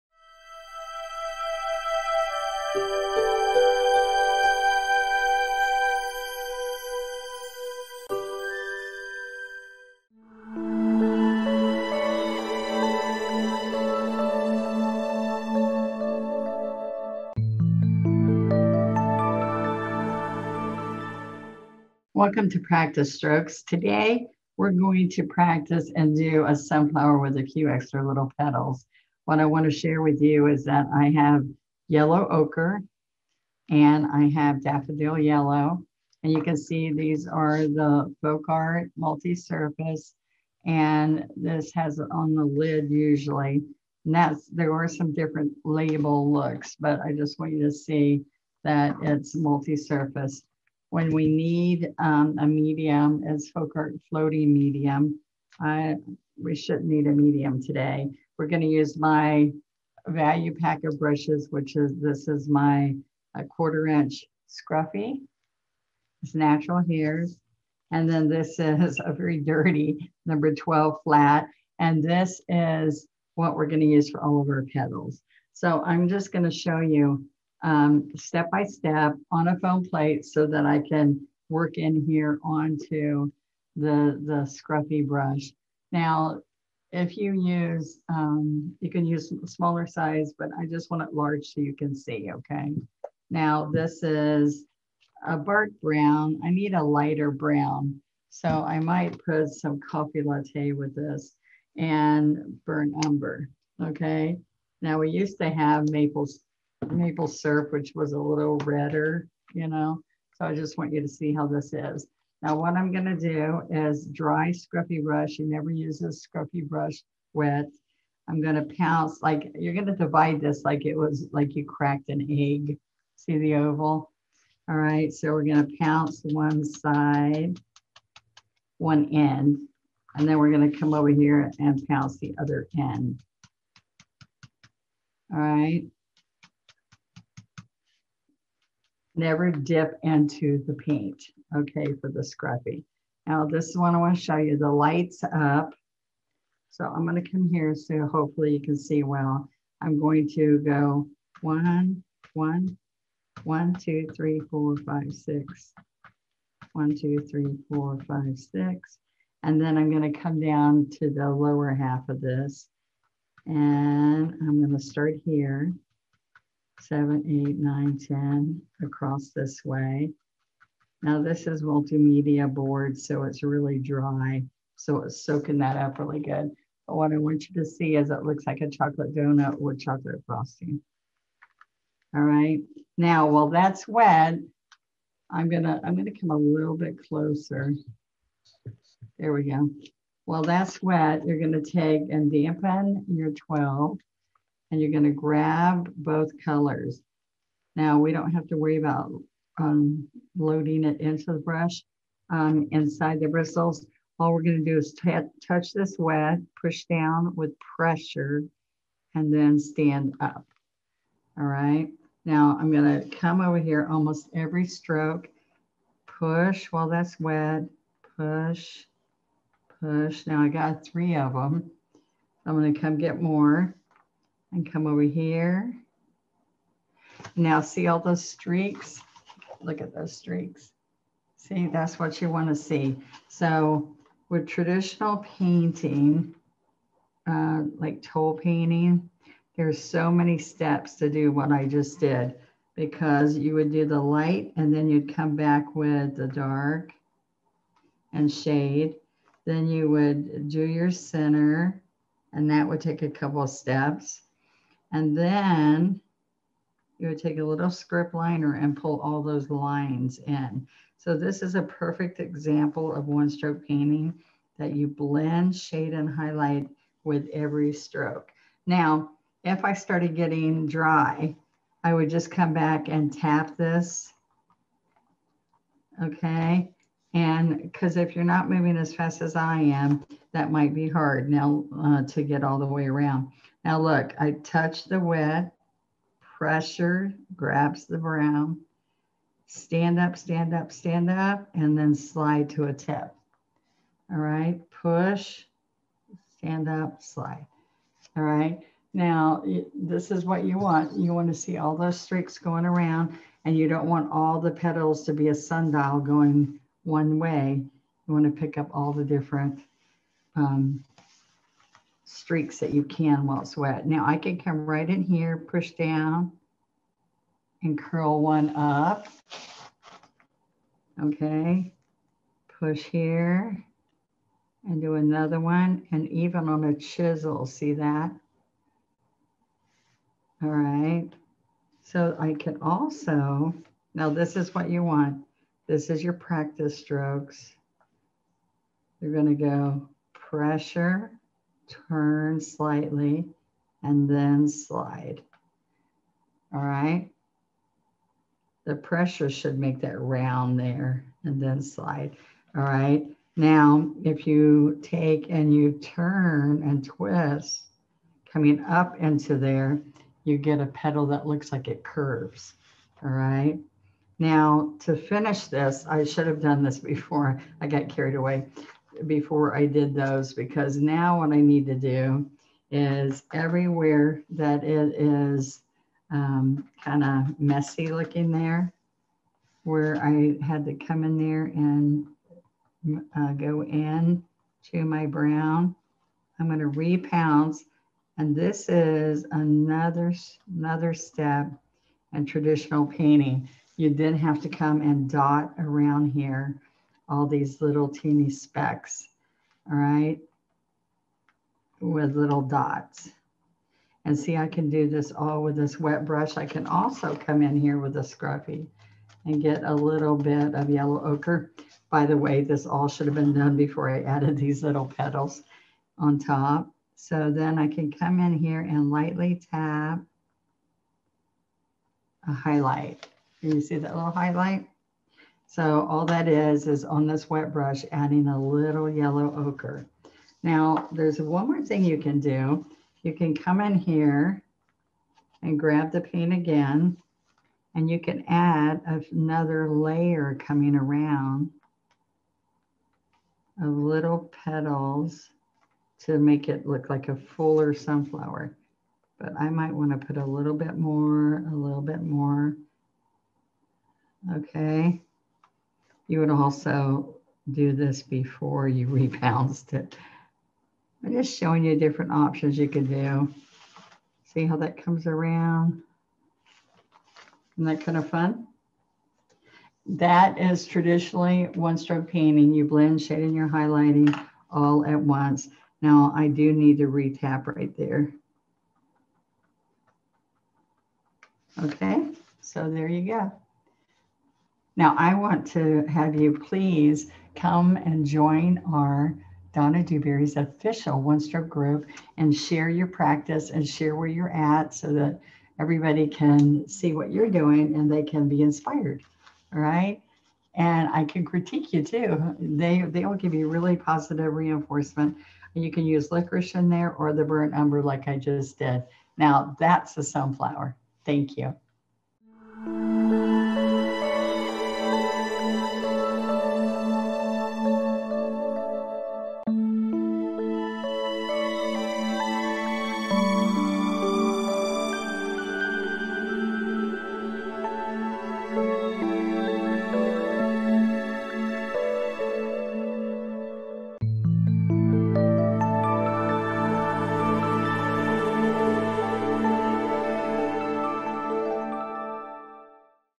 FolkArt One Stroke Practice Strokes With Donna - Layered Petal Sunflower | Donna Dewberry 2021
Practice One Stroke Painting with me - Donna Dewberry.  Today I share one of my favorites - a large Sunflower with layers of petals, even a few laying over the center!

Comment on whether you like this format and want to see more!